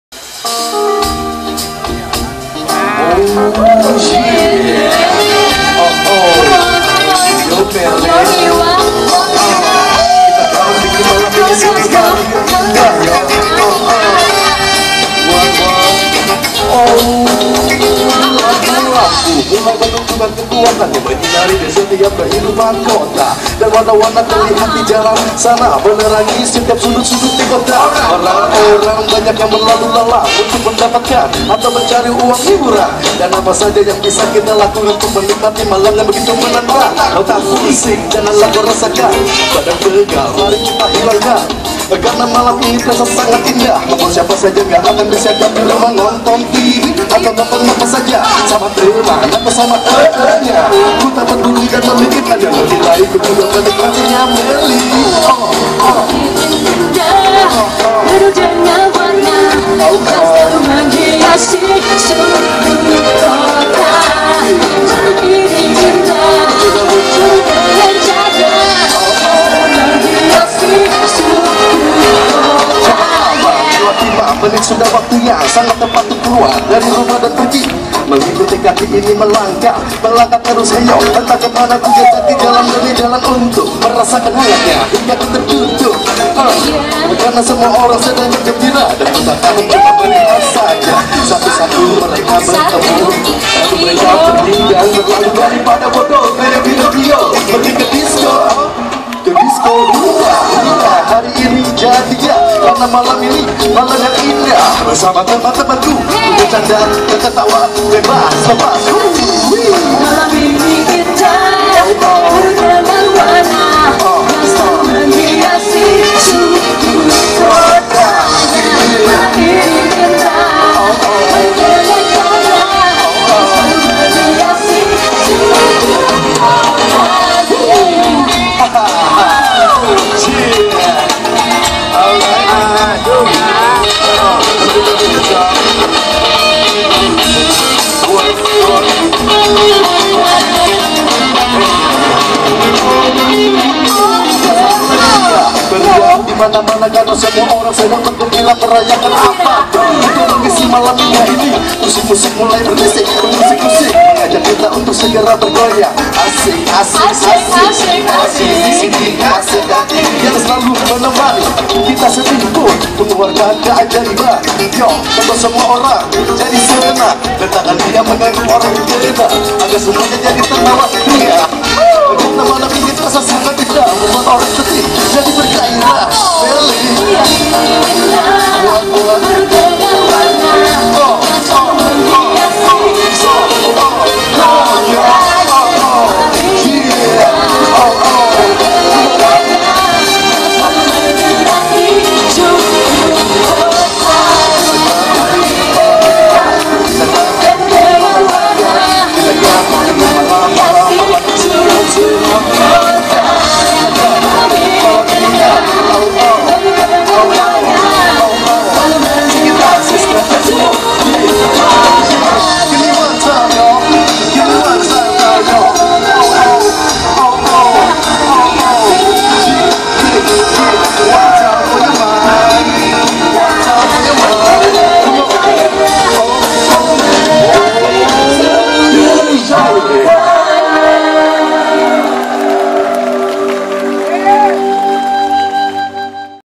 Closing Ceremony Kongres Partai NASDEM 3 by Propile Dance Project
Closing Ceremony Kongres Partai NASDEM
Balai Sidang Jakarta Convention Center
by Profile Dance Project